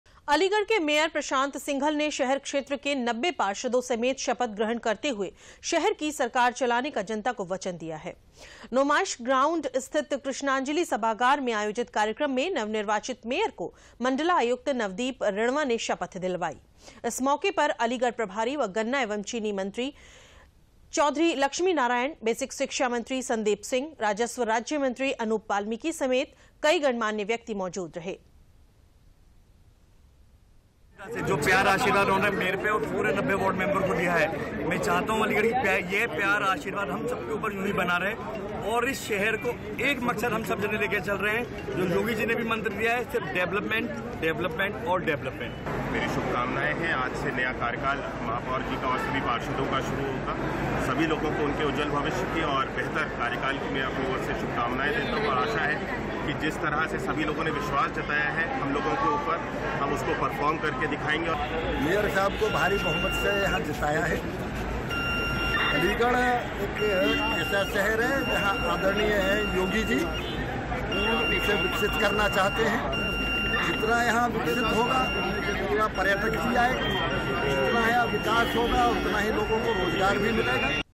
Aligarh के मेयर प्रशांत सिंघल का शपथ ग्रहण समारोह सम्पन्न
अलीगढ़ के मेयर प्रशांत सिंघल ने शहर क्षेत्र के 90 पार्षदों समेत शपथ ग्रहण करते हुए शहर की सरकार चलाने का जनता को वचन दिया है।  नुमाइश ग्राउंड स्थित कृष्णाजिली सभागार में आयोजित कार्यक्रम में नवनिर्वाचित मेयर को मंडलायुक्त नवदीप रिणवा ने शपथ दिलाई। इस मौके पर अलीगढ़ प्रभारी व गन्ना एवं चीनी मिल मंत्री चौधरी लक्ष्मी नारायण, बेसिक शिक्षा मंत्री संदीप सिंह, राजस्व राज्य मंत्री अनूप वाल्मीकि समेत कई गणमान्य व्यक्ति मौजूद रहे।